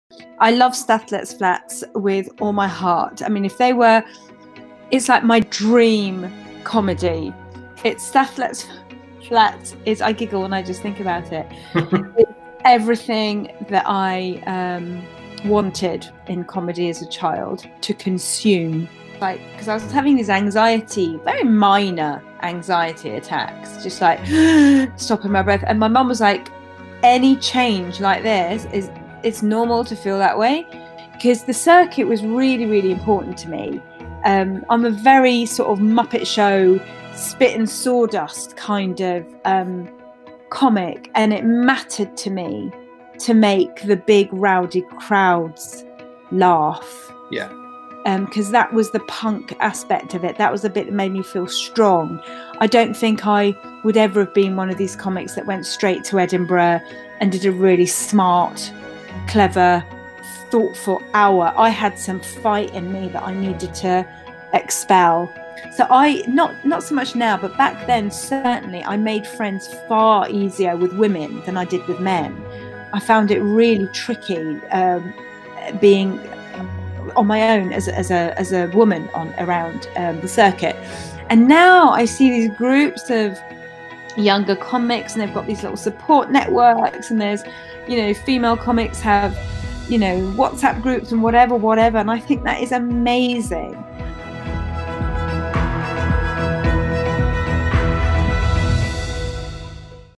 An Interview with Shappi Khorsandi
Leicester Comedy Festival director Geoff Rowe will be interviewing Shappi Khorsandi on Thursday the 17th of September 2020. They will talk about Shappi's life in comedy, what she's been up to in lockdown and much more.

This exclusive interview will be available for viewing in aid of Leicester Comedy Festival and the charity Big Difference Company.

You can donate by visiting to support the UK's biggest comedy festival.